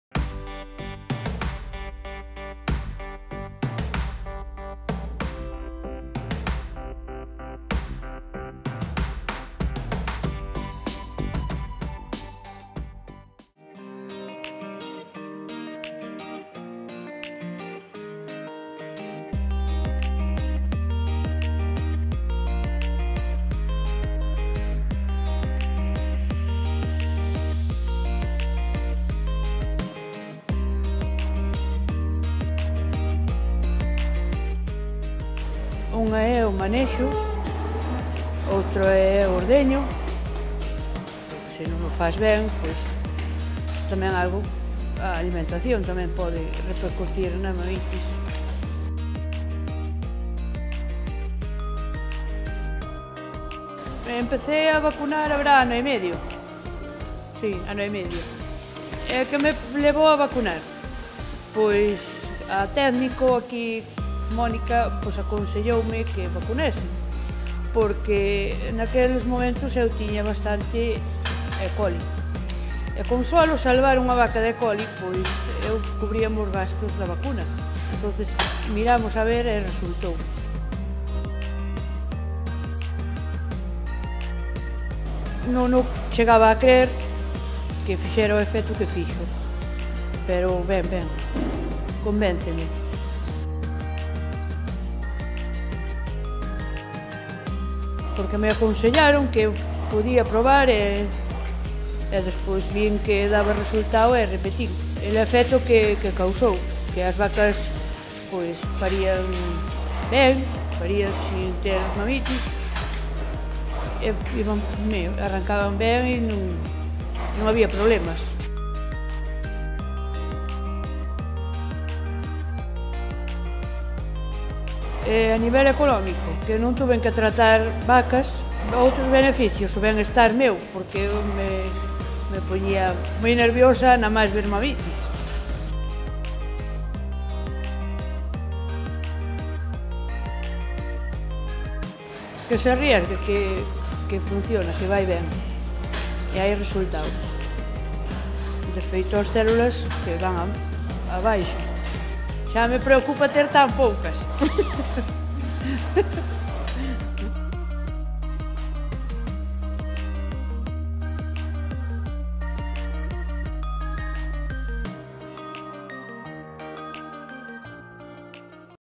Vaccination against Mastitis. Interview to María del Carmen, partner of Veiga da Lama
En la ganadería Veiga da Lama (Castro de Rei, Lugo) empezaron a vacunar a los animales contra la mastitis hace aproximadamente un año y medio. María del Carmen Val Campo, socia de la explotación, y Mónica Mon, veterinaria especialista en Calidad de Leche, responden a unas preguntas sobre la experiencia con esta vacuna desarrollada por Hipra.

LABORATORIOS HIPRA, S.A. © - 2017